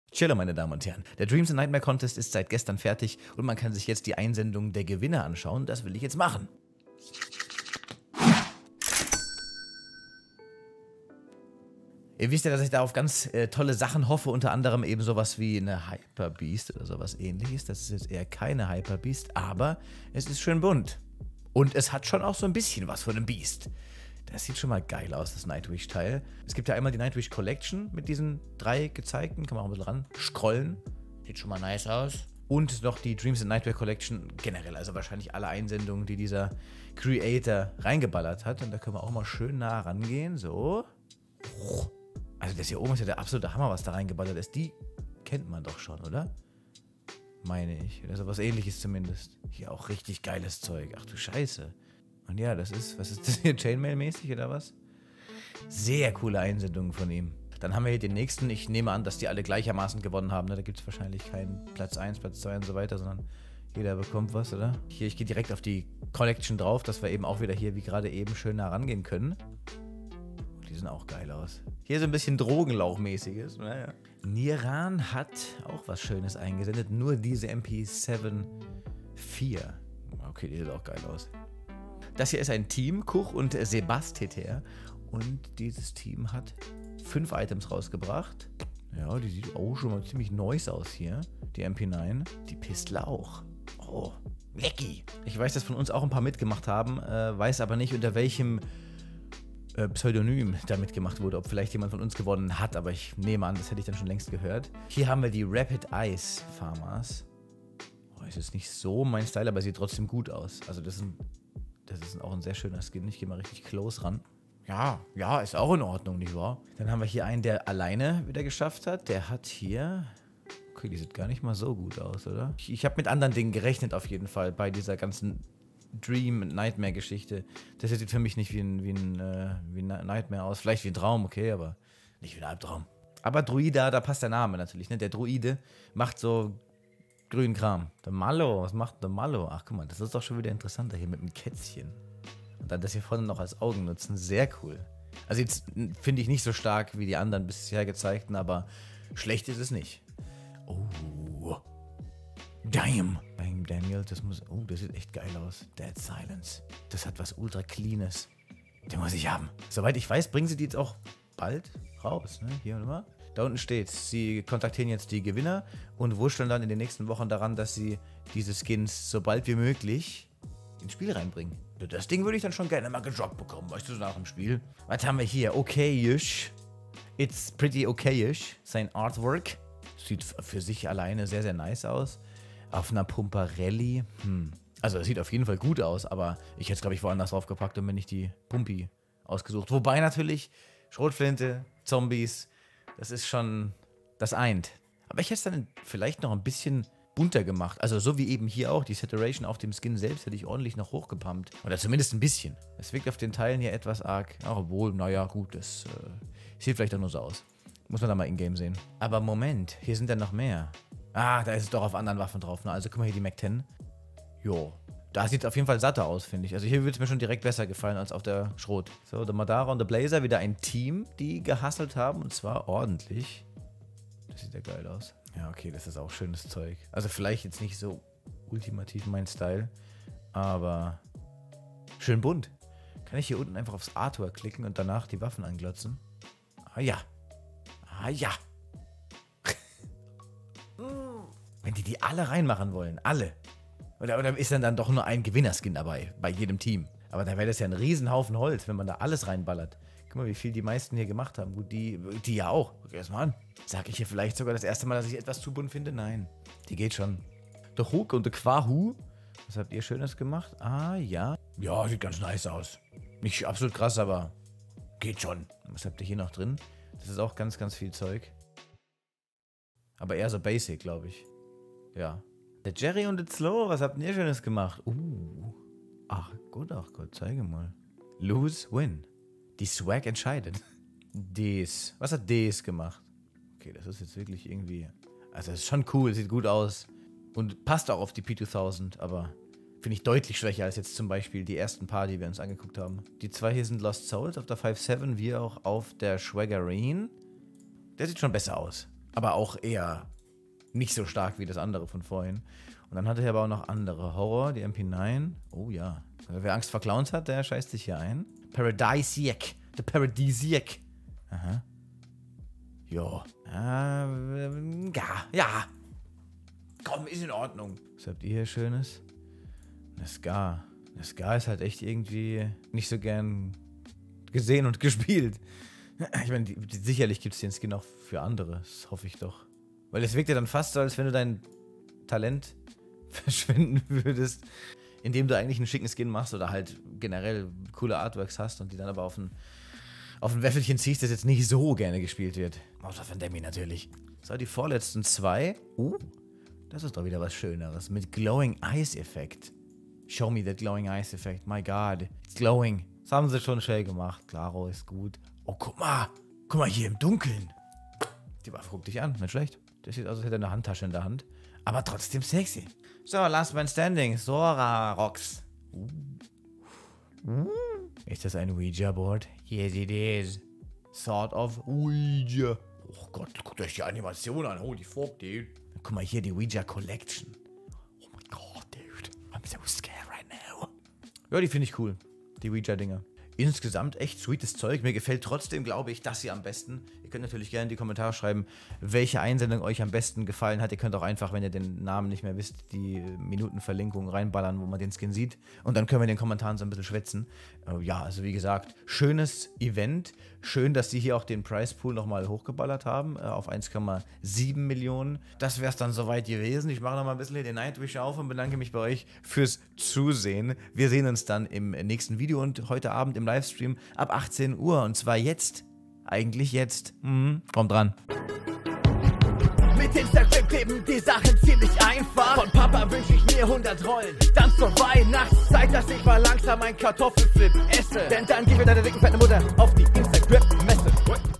Vorfreude auf die Neue CS:GO Case! | CS:GO Dreams & Nightmares Gewinner
Die CS:GO Dreams & Nightmares Gewinner sind endlich da! Schauen wir uns mal die neuen Skins an, die mit hoher Wahrscheinlichkeit eine neue CS:GO Case "werden".

LIVESTREAM, VIEWERGAMES & Skingiveaways jeden Di und Do ab 18 Uhr Uhr und jeden Freitag ab 20 Uhr!

• Mein Epic Creator Code: DERDUMMBABBLER